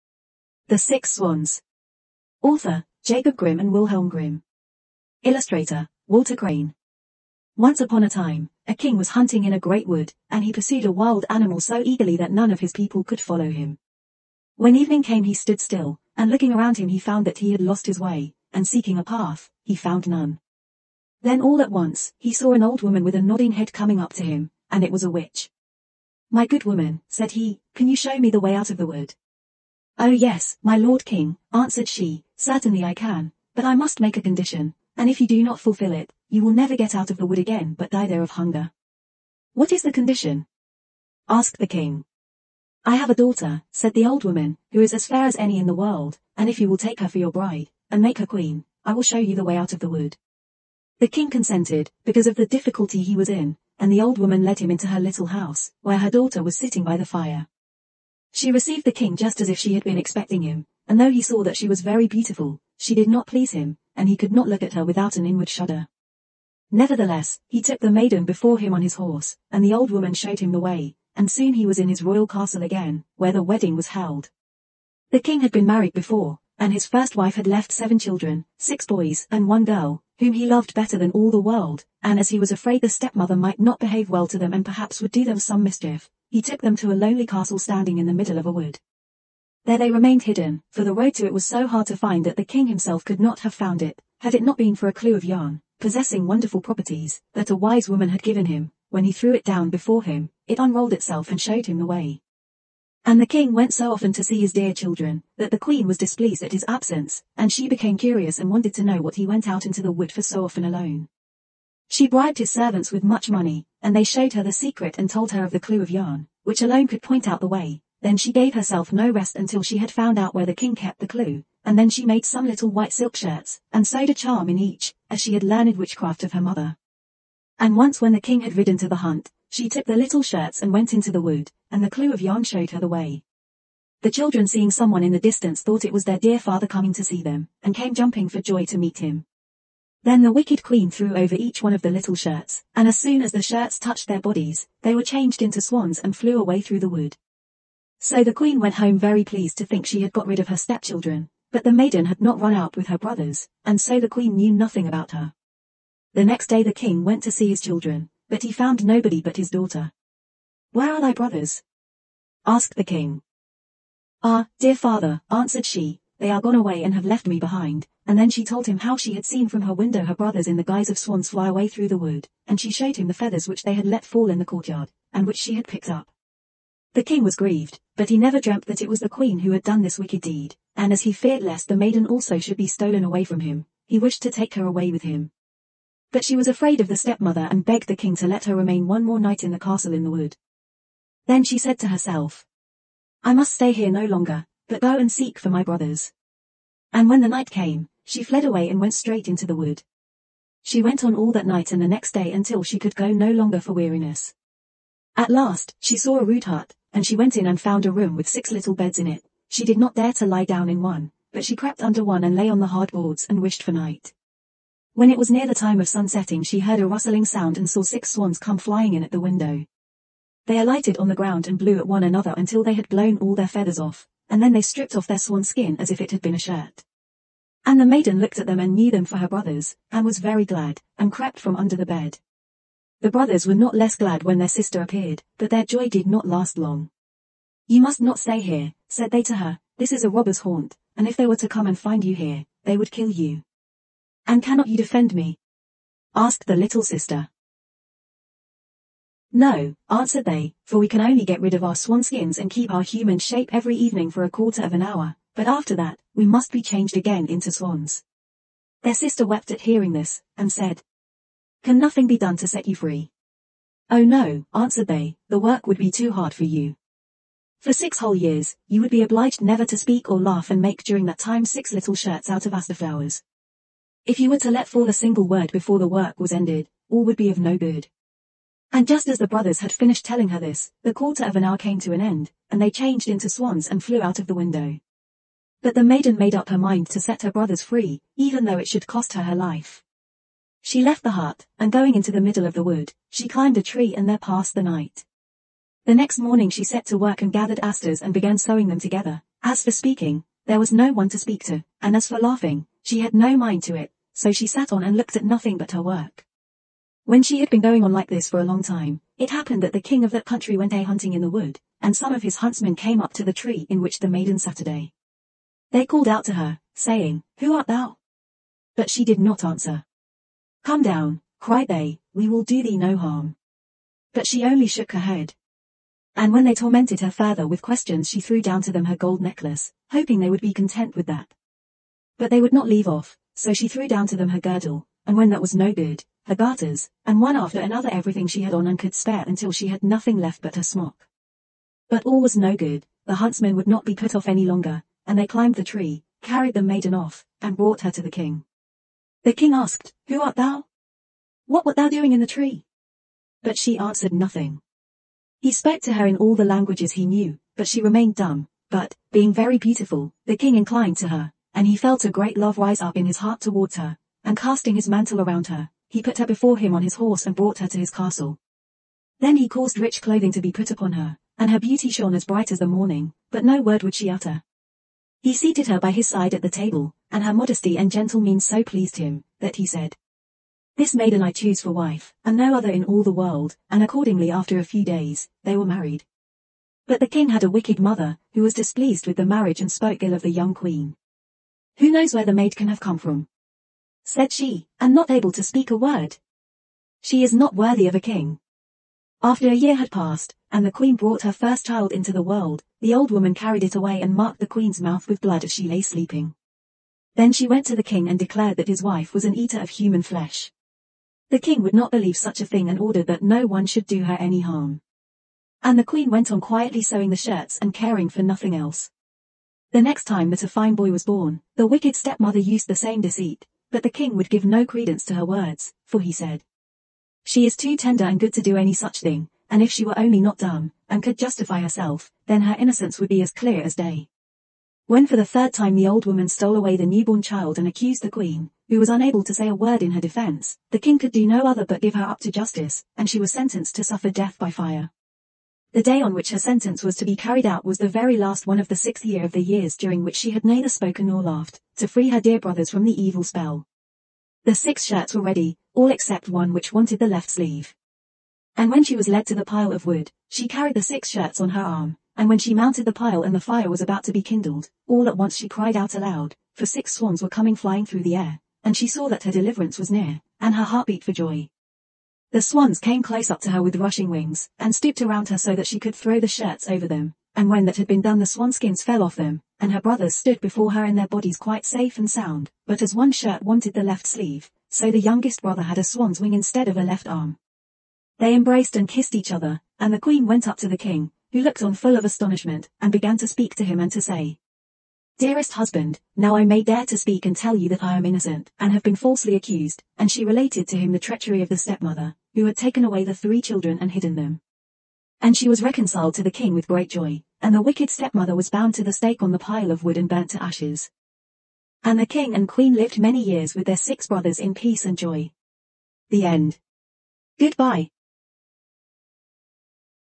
THE SIX SWANS Author: Jacob Grimm and Wilhelm Grimm SE60
Once upon a time, a king was hunting in a great wood, and he pursued a wild animal so eagerly that none of his people could follow him. When evening came he stood still, and looking around him he found that he had lost his way, and seeking a path, he found none. Then all at once, he saw an old woman with a nodding head coming up to him; and it was a witch.
"My good woman," said he, "can you show me the way out of the wood?"
"Oh yes, my lord king," answered she, "certainly I can; but I must make a condition, and if you do not fulfil it, you will never get out of the wood again but die there of hunger."
"What is the condition?" asked the king.
"I have a daughter," said the old woman, "who is as fair as any in the world, and if you will take her for your bride, and make her queen, I will show you the way out of the wood."
The king consented, because of the difficulty he was in, and the old woman led him into her little house, where her daughter was sitting by the fire.

She received the king just as if she had been expecting him, and though he saw that she was very beautiful, she did not please him, and he could not look at her without an inward shudder. Nevertheless, he took the maiden before him on his horse, and the old woman showed him the way, and soon he was in his royal castle again, where the wedding was held.
The king had been married before, and his first wife had left seven children, six boys and one girl, whom he loved better than all the world, and as he was afraid the step-mother might not behave well to them and perhaps would do them some mischief, he took them to a lonely castle standing in the middle of a wood.